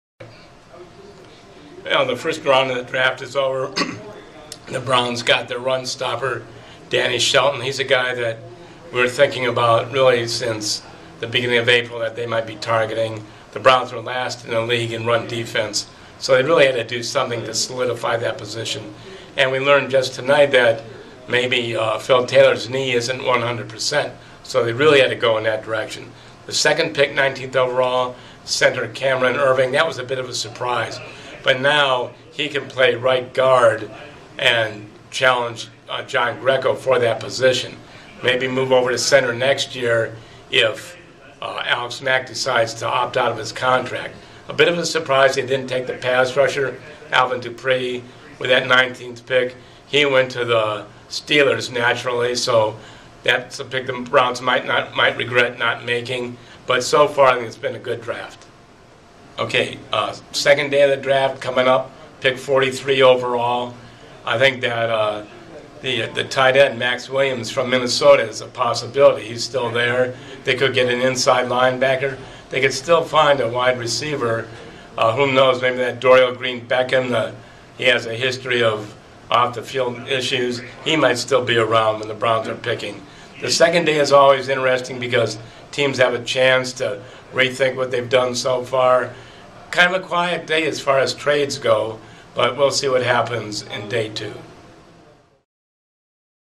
News-Herald Browns beat writer Jeff Schudel talks about Day 1 of the draft and takes a look what Day
News-Herald Browns beat writer Jeff Schudel talks about Day 1 of the draft and takes a look what Day 2 might hold for the Browns.

By: John Kampf - My name is John Kampf. I cover high school sports and Ohio State football for The News-Herald.